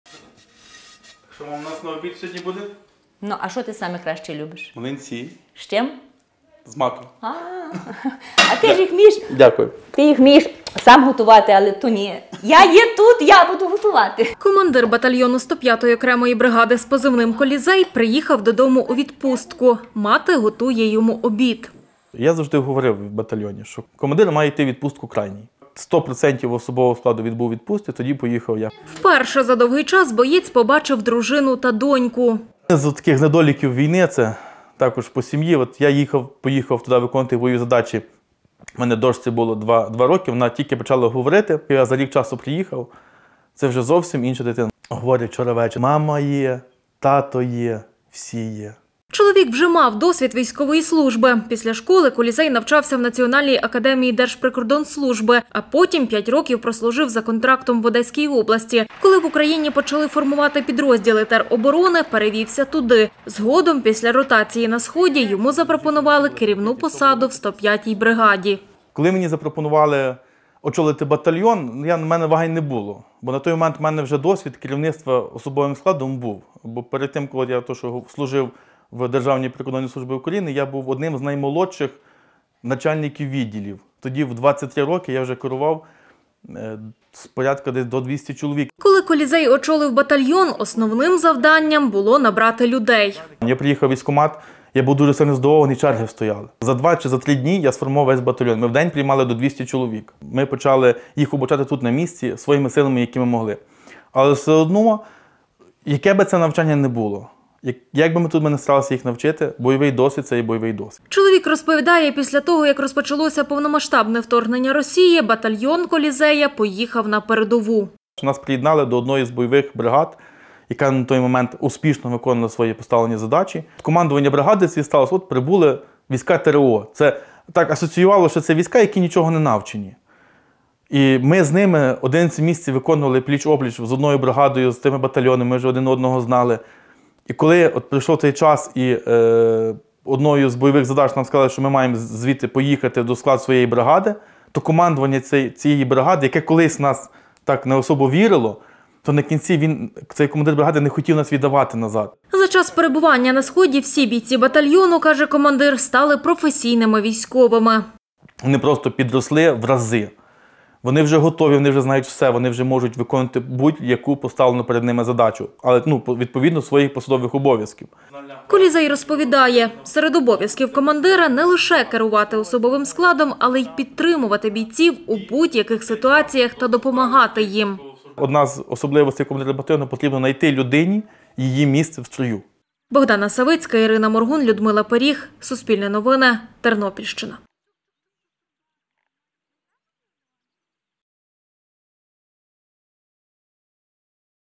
Історія командира батальйону з Тернопільщини з позивним «Колізей»
Боєць з позивним "Колізей" – командир батальйону 105 окремої бригади.  Вперше за довгий час боєць повернувся з фронту додому. Вдома на нього чекала дружина, маленька донечка та матір. Не зважаючи на відпустку, чоловік продовжує тримати зв'язок зі своїми підлеглими. Каже – командир завжди повинен бути поруч з бойовим складом. Історію "Колізея" дивіться в сюжеті.